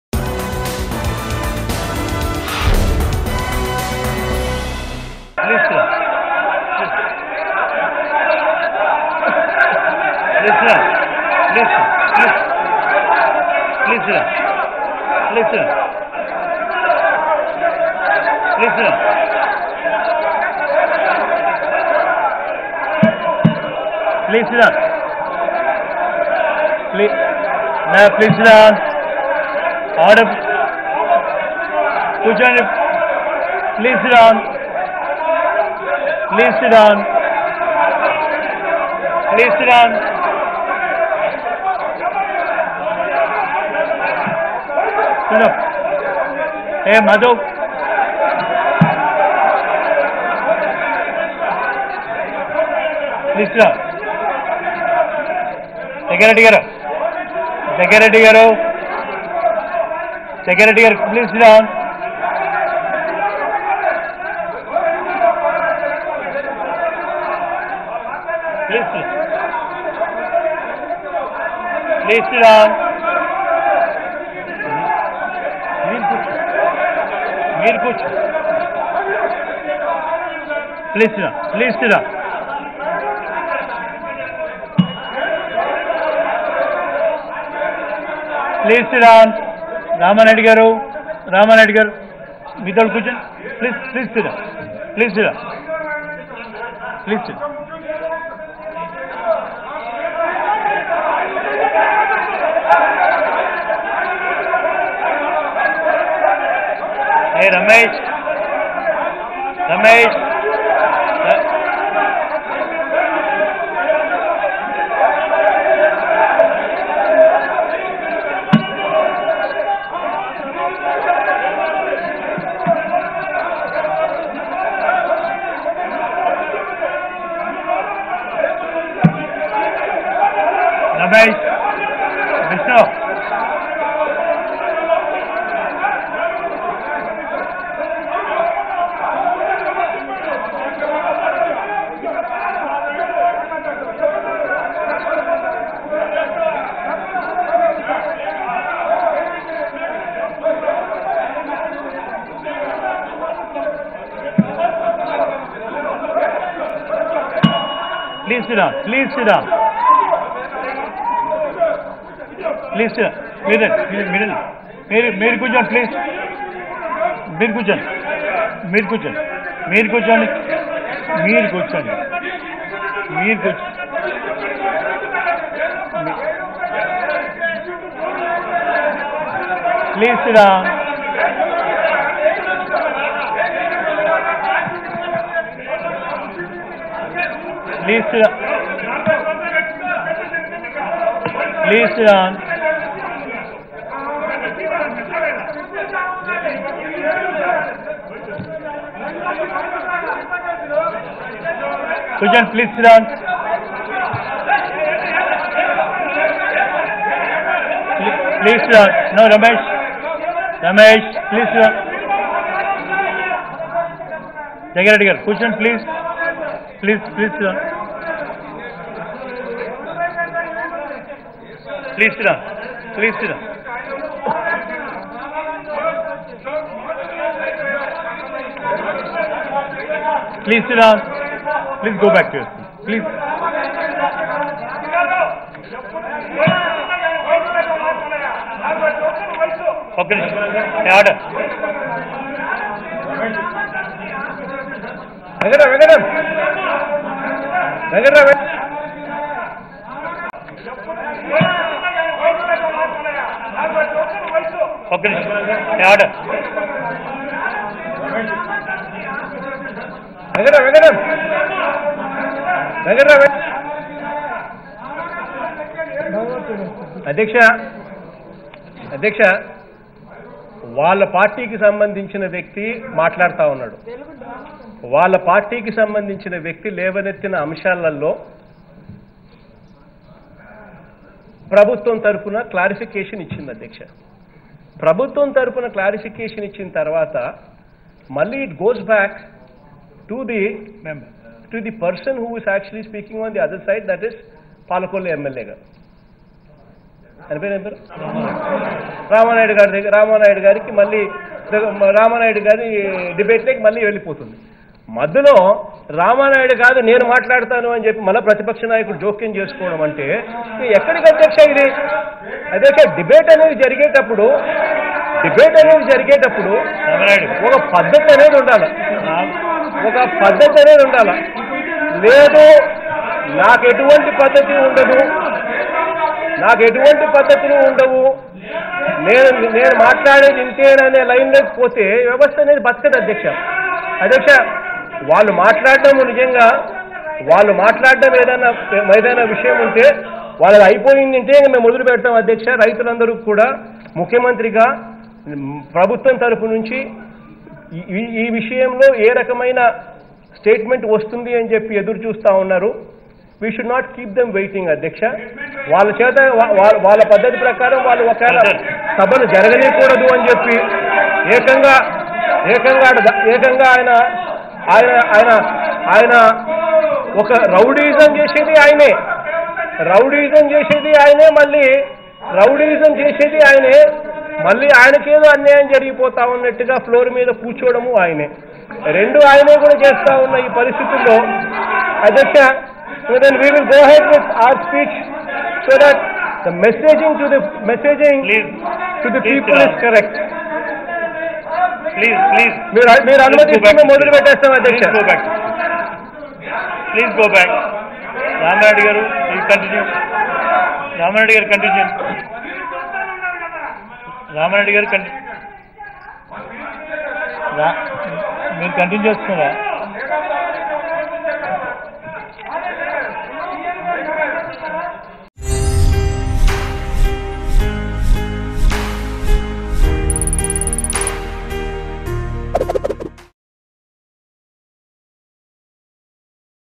CM YS Jagan Laughs At Chandrababu Naidu In AP Assembly Session | AP Latest Updates | Mango News
CM YS Jagan Laughs At Chandrababu Naidu In AP Assembly Session.